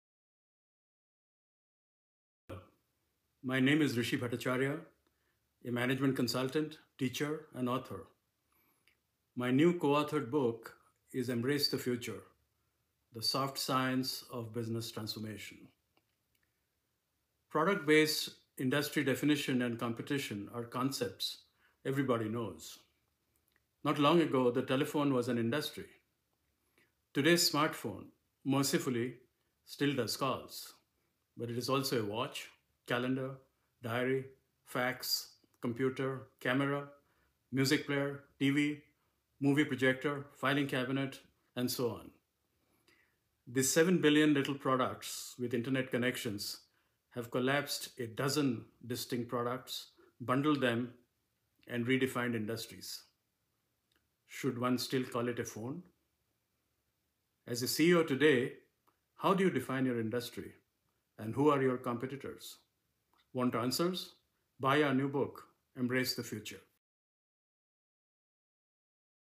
Defining an industry in the future with Hrishi Bhattacharya
In this video, Hrishi Bhattacharya delivers a breakthrough analysis on defining an industry. Discover exclusive insights and future predictions as we delve into the realm of tomorrow's industries. Stay ahead of the curve with expert guidance with our new book, 'Embrace the Future: The Soft Science of Business Transformation'.